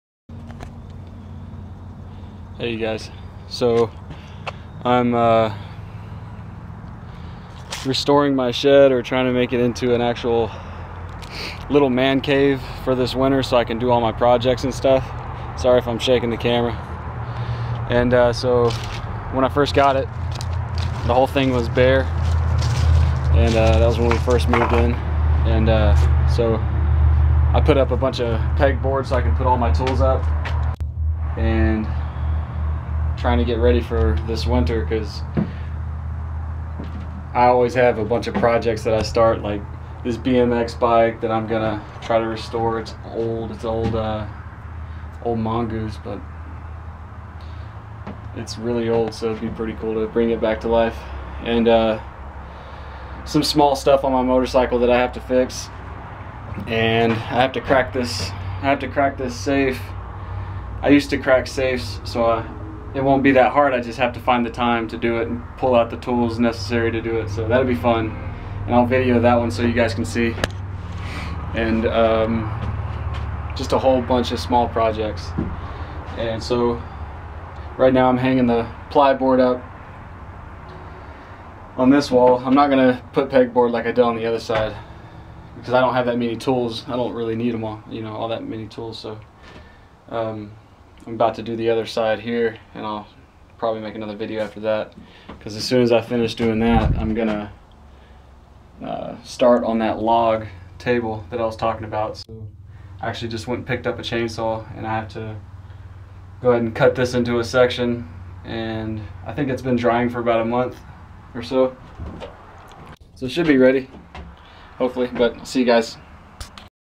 Man cave shed creation #1
A quick peek at  my shed before it's turned into the man cave.

Man cave, vlog, update, winter projects.